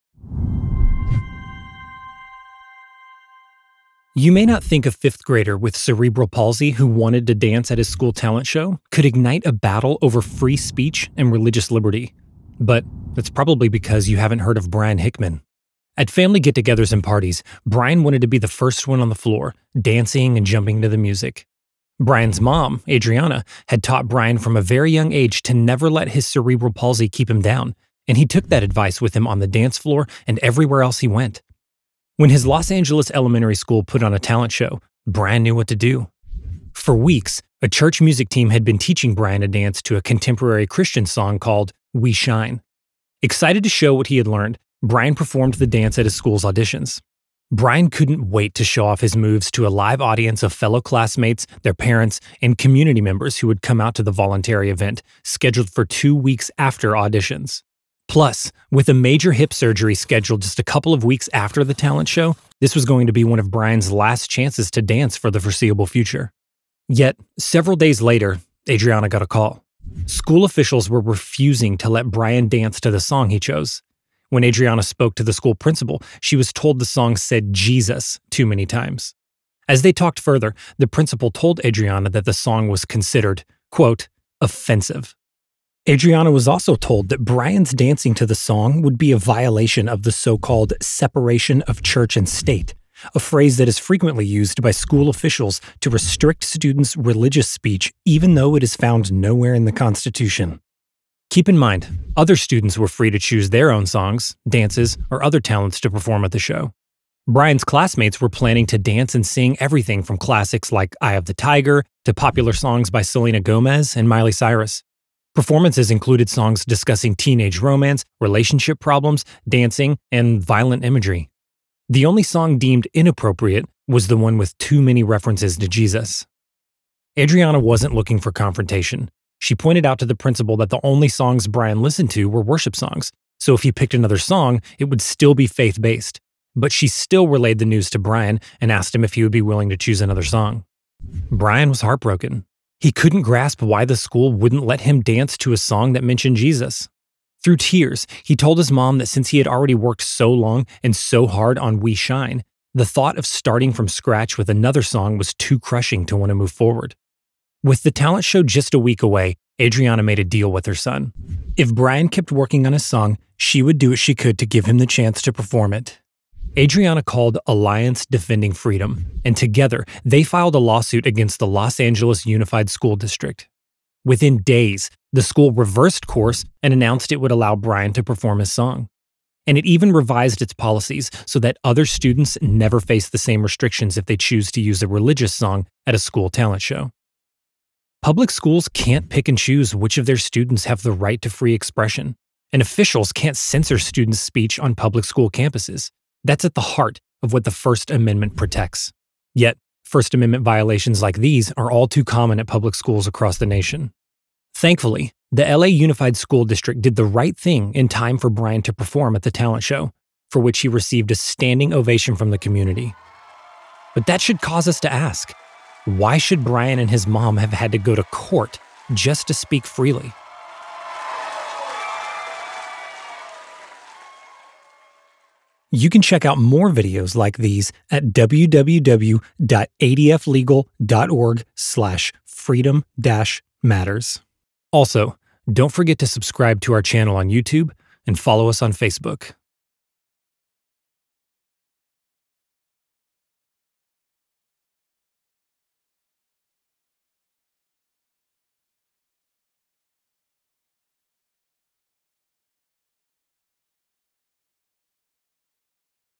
How Free Speech Took Center Stage at a School Talent Show
Brian Hickman loves to dance. His mom, Adriana, taught him to never let his cerebral palsy keep him down, and he took that advice with him everywhere he went.

So when Brian’s elementary school announced they were going to have a student talent show, the fifth-grader was excited to showcase his dancing talents in front of his fellow classmates. He spent weeks practicing a dance routine he learned at church, working hard to nail down all the right moves.

But a week before the talent show, Brian’s mom received a phone call from the school’s principal. He told her that the song Brian had chosen “said Jesus too many times,” and violated the so-called “separation of church and state”--a phrase found nowhere in the Constitution. He would have to select a different song, or not perform in the school’s talent show.

Brian was heartbroken. He worked hard to master his routine, and couldn’t understand what was so controversial about dancing to a song about Jesus.

With the talent show just a week away, Adriana made a deal with her son. If Brian kept working on his song, she would do what she could to give him the chance to perform his chosen song. Adriana called Alliance Defending Freedom, and together, they filed a lawsuit against the Los Angeles Unified School District.

Within days, the school reversed course and announced it would allow Brian to perform his song. And it even revised its policies so that other students never face the same restrictions if they choose to use a religious song at a school talent show.

Thankfully, the LA Unified School District did the right thing in time for Brian to perform at the talent show—for which he received a standing ovation from the community. But that should cause us to ask, “Why should Brian and his mom have had to go to court just to speak freely?”